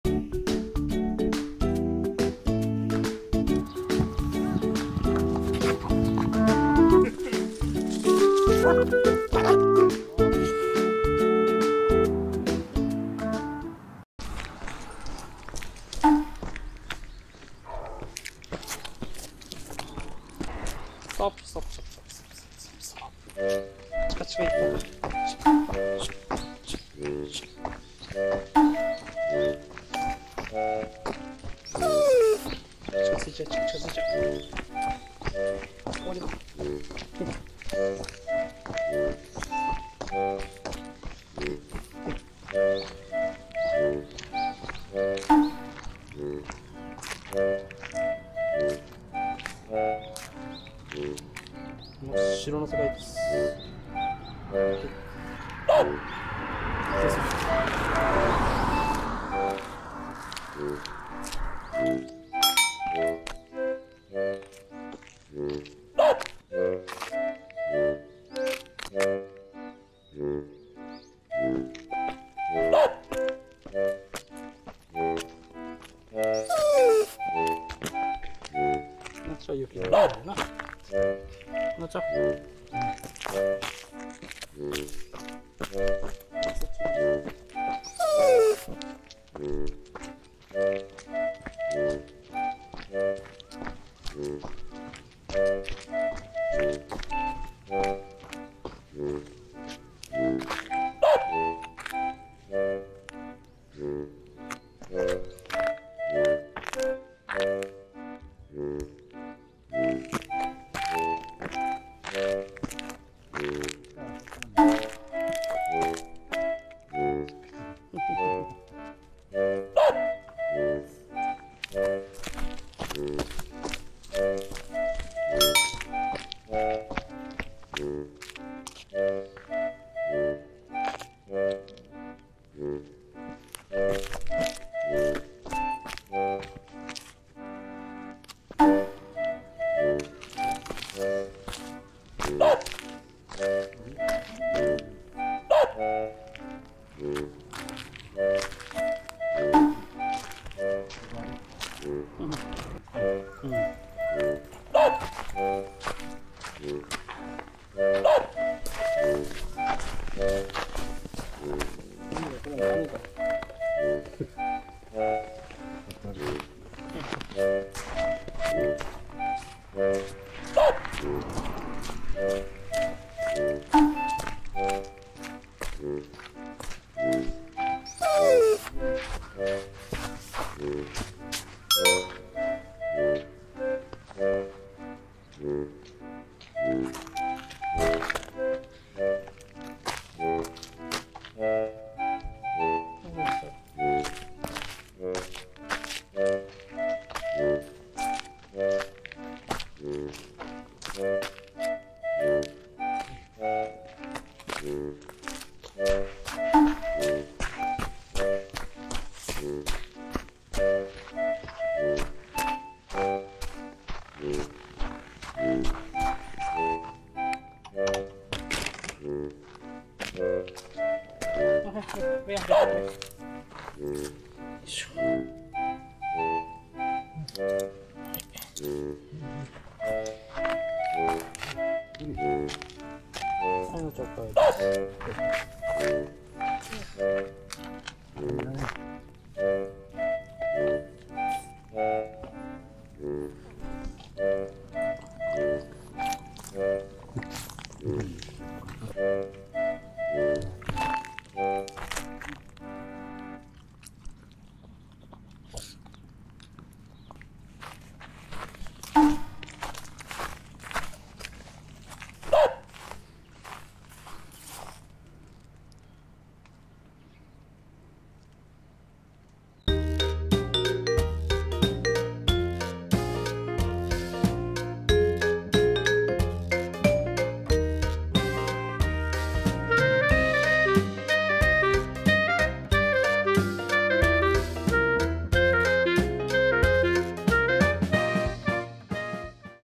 178『うっすら真っ白散歩編』ベリとナチャ【ほっこりペキニーズ】
一緒に暮らすと病みつきになる
ペキニーズ2匹の動画です。

第178回は畑の真っ白な世界に直面したベリナチャ。
二人は何を思ったのか…

その他、関連チャンネルもありますので